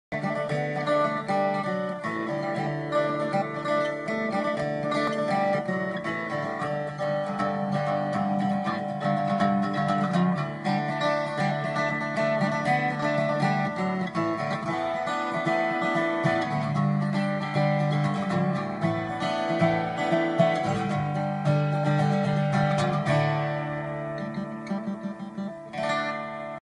Hank's Blog 03
Recording dobros. Just jamming a little bit